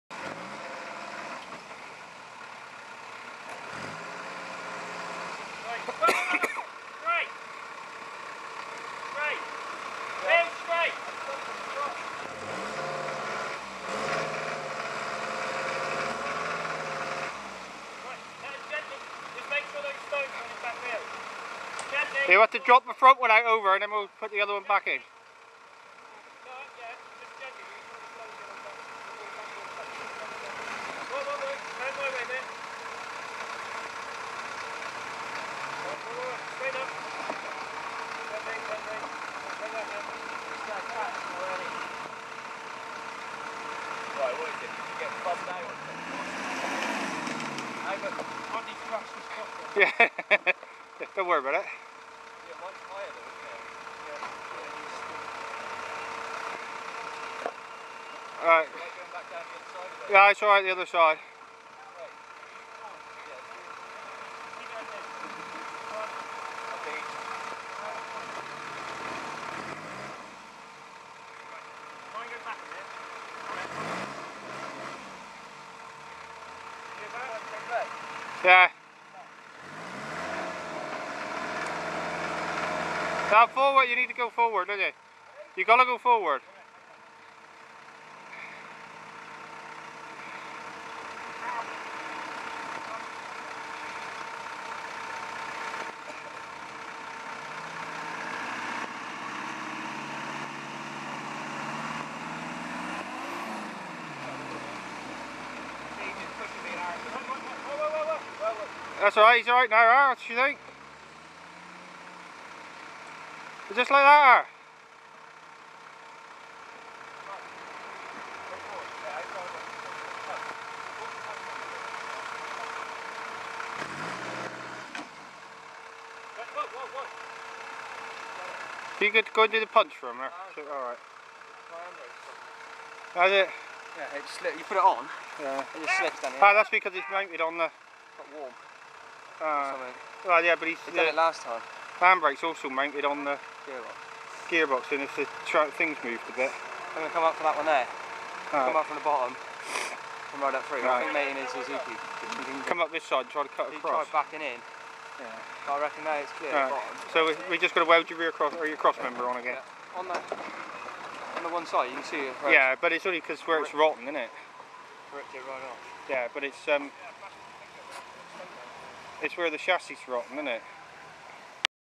Tom Marsh Series 3 Pure engine sound.
Tom marsh in his series 3 first time off road